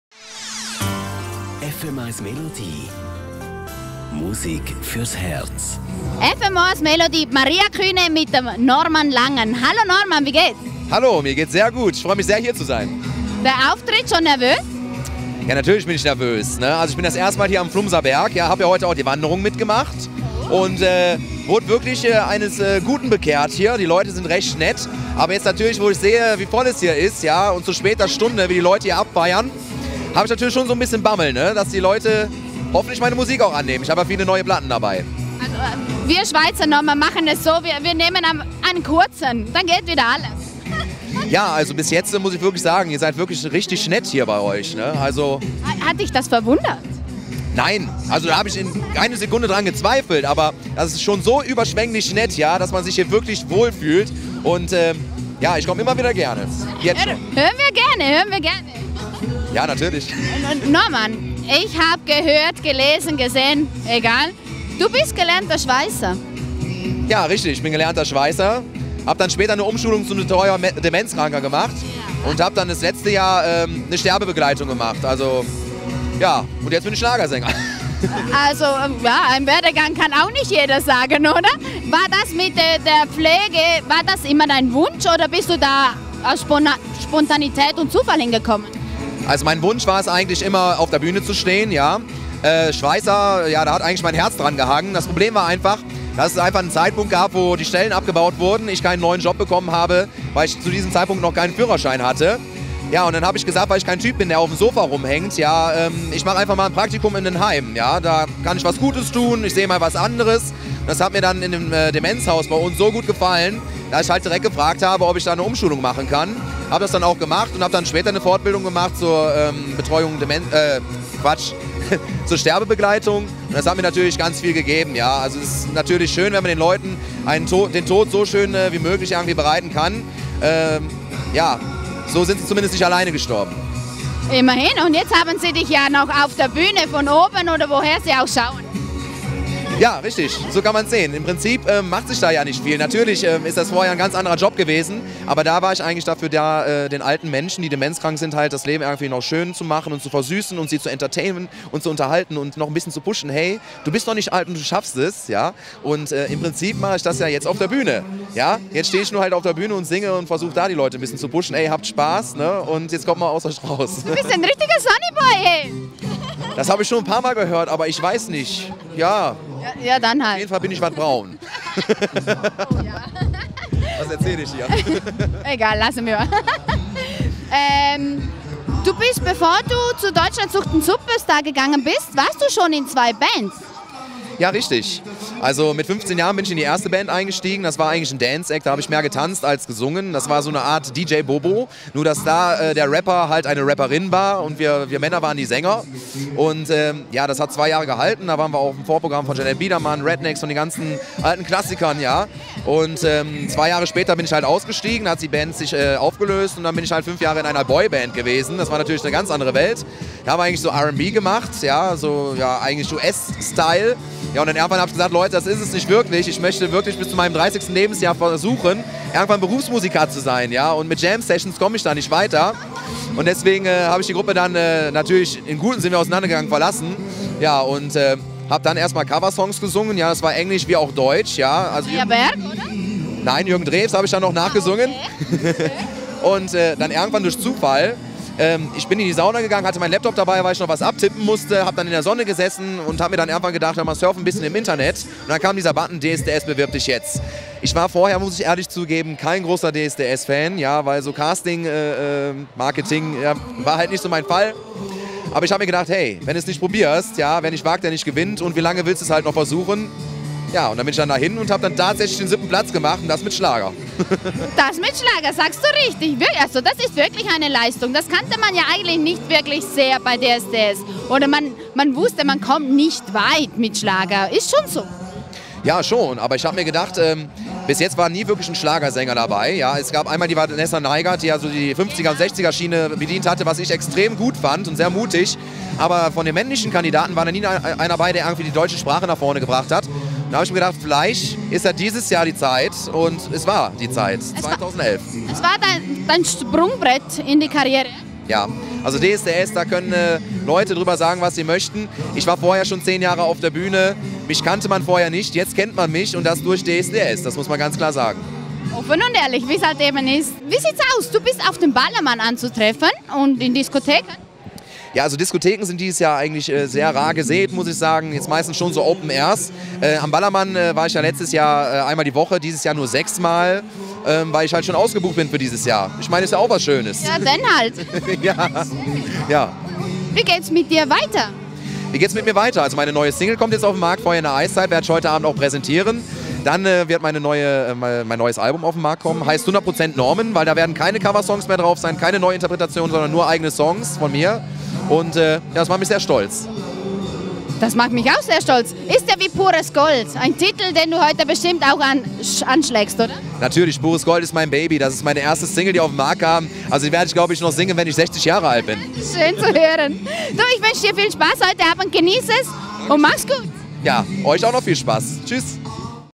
Norman Langen im Radio Melody Interview
Norman Langen im Radio Melody Interview mit Maria Kühne kurz vor dem Auftritt am Schlager Open Air 2012 am Flumserberg.